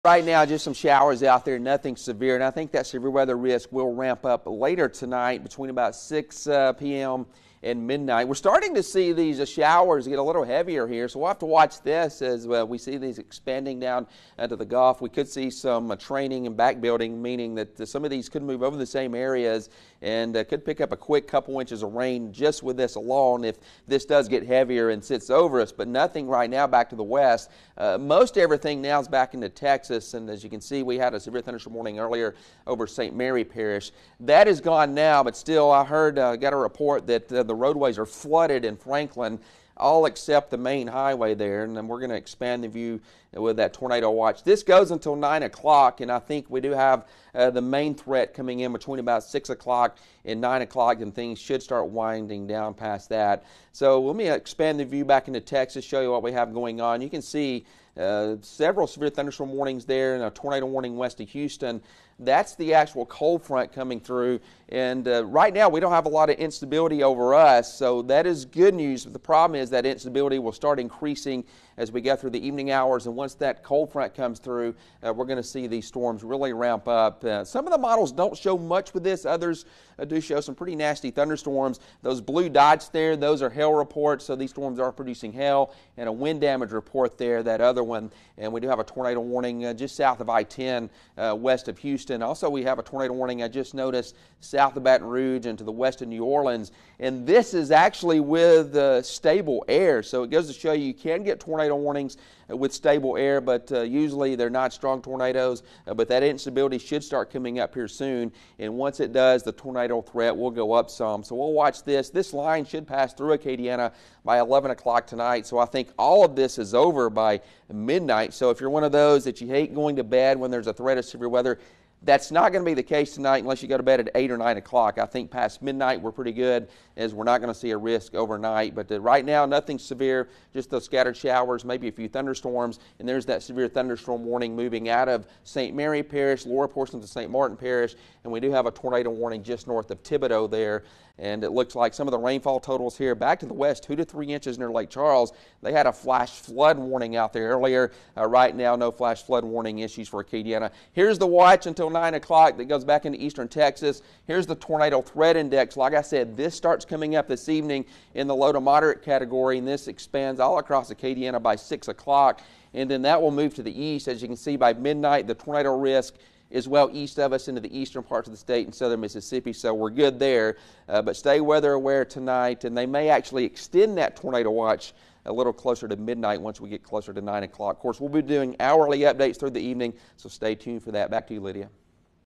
Tornado Watch issued for Acadiana
A Tornado Watch has been issued for Acadiana. This includes the Acadia, Vermilion, Lafayette, Evangeline, St. Mary, Iberia, Jefferson Davis, St. Landry and St. Martin parishes until 9 p.m.